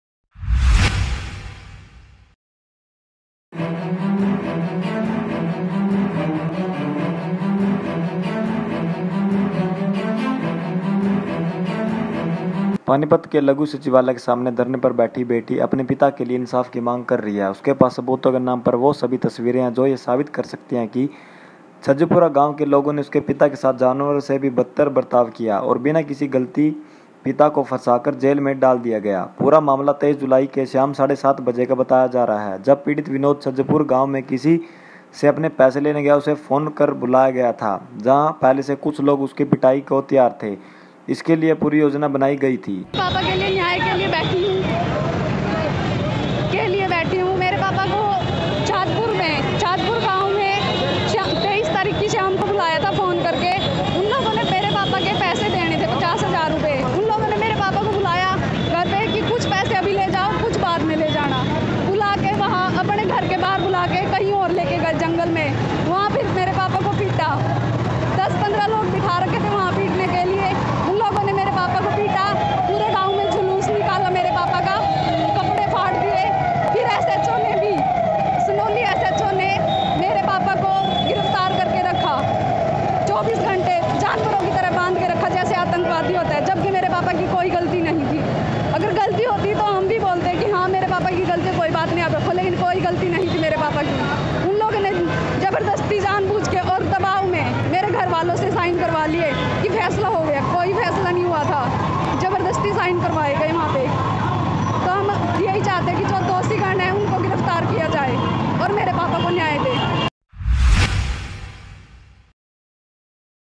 जब दबंगों ने पिता को तांत्रिक बताकर बुरी तरह पीटा, तो इंसाफ के लिए बेटी धरने पर बैठ गई
पानीपत के लघुसचिवालय के सामने धरने पर बेटी, अपने पिता के लिए इंसाफ की मांग कर रही है, उसके पास सबूतों के नाम पर वो सभी तस्वीरें हैं, जो ये साबित कर सकती हैं, कि छाजपुर गांव के लोगों ने उसके पिता के साथ जानवरों से भी बदतर बर्ताव किया, और बिना किसी गलती, पिता को फंसाकर जेल में डाल दिया गया। पूरा मामला 23 जुलाई के शाम 7:30 बजे का बताया जा रहा है, जब पीड़ित विनोद छाजपुर गांव में किसी से अपने पैसे लेने गया, उसे फोन कर बुलाया गया था, जहां पहले से कुछ लोग उसकी पिटाई को तैयार थे, इसके लिए पूरी योजना बनाई गई थी।